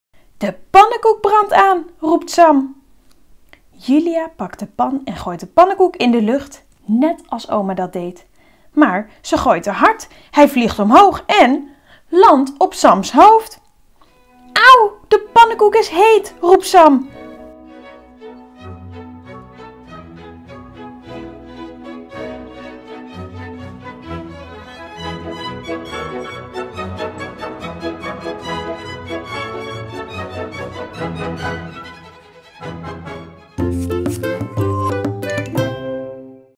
Het Muizenhuis - Sam en Julia: 3. Pannenkoeken - De Voorleeshoek
Het Muizenhuis - Sam en Julia: 3. Pannenkoeken
Auteur: Karina Schaapman
Foto's: Ton Bouwer
Uitgeverij: Rubinstein

De Voorleeshoek is een online videoplatform voor kinderen van 0-10 jaar. Benieuwd naar het volledige voorleesverhaal? Klik hier voor een eerste maand gratis voorleesverhalen: